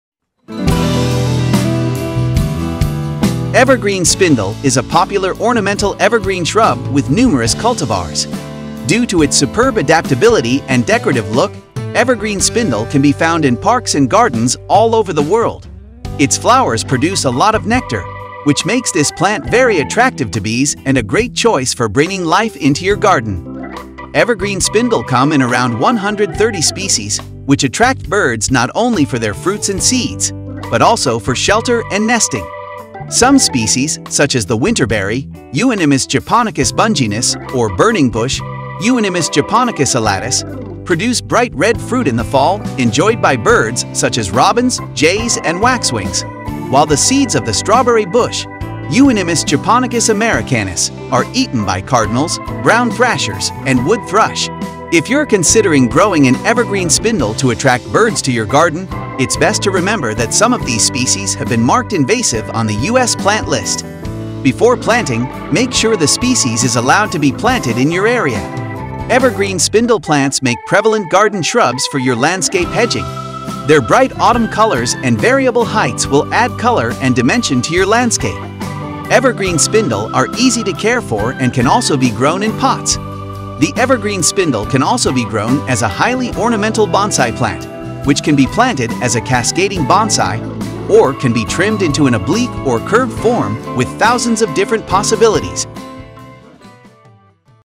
Foresight, mysticism--Evergreen spindle
Evergreen spindle(Euonymus japonicus) is a popular ornamental evergreen shrub&nbsp;with&nbsp;numerous cultivars. Due to its superb adaptability and decorative looks, evergreen spindle can be found in parks and gardens all over the world. Its flowers produce a lot of nectar, which makes this plant very attractive to bees.